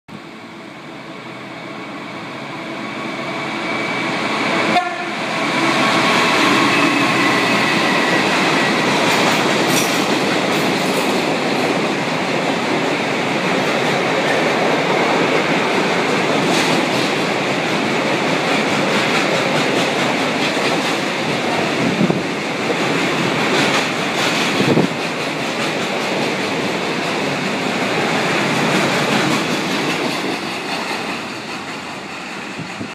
EWS class 66 130 horn @ Cheltenham Spa 30/10/15
EWS class 66 130 passes Cheltenham Spa on steel working between margam and corby on the 30th of October 2015.